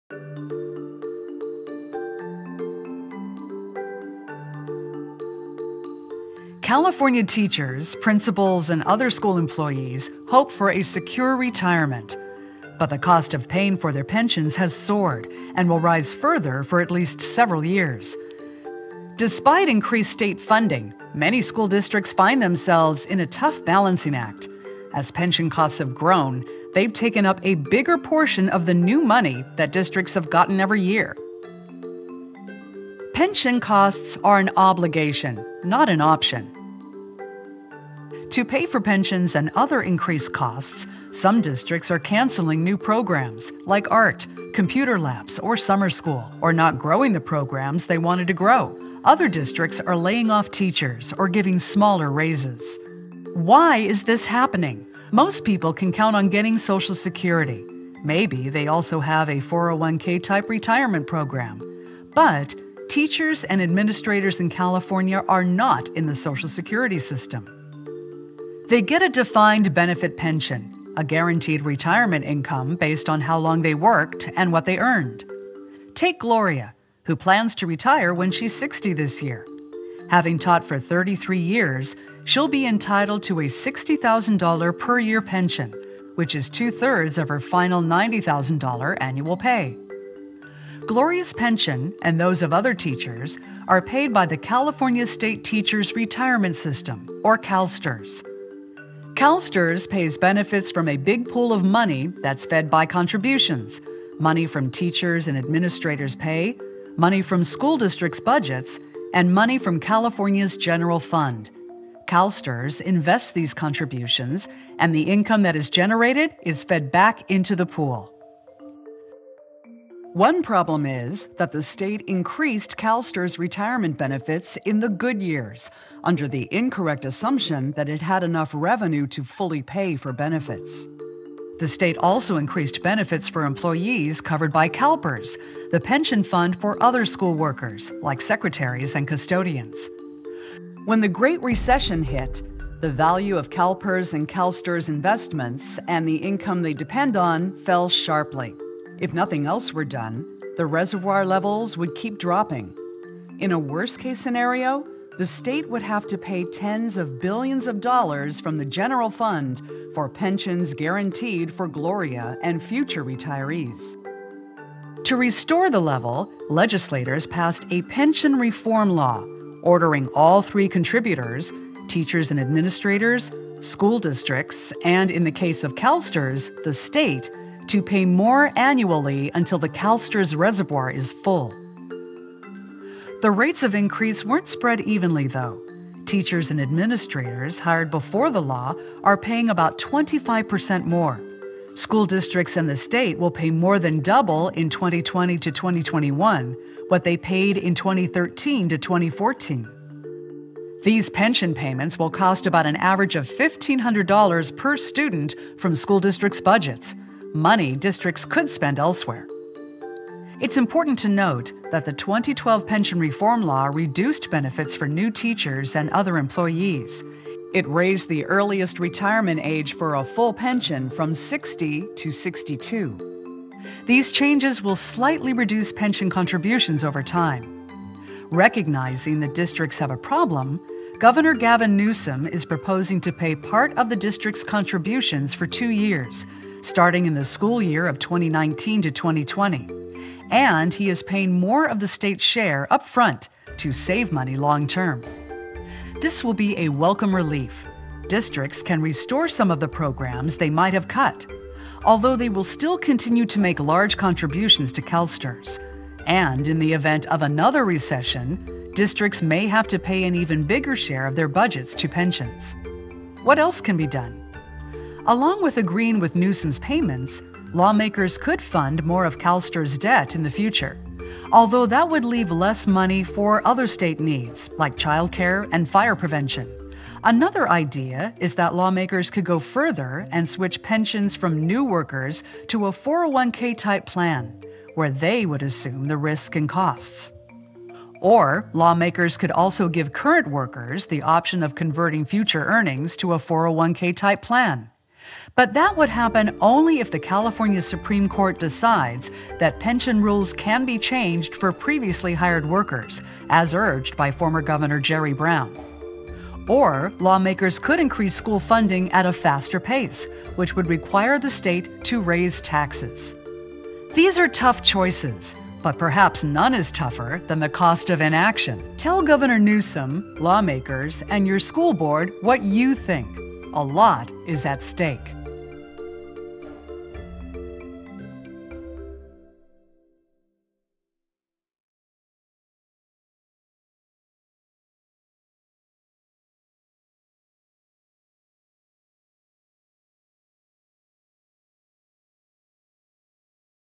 Explainer: Schools and Pension Costs in California
This video explainer from EdSource sheds light on the challenges facing California as pension obligations take up a growing share of school district budgets.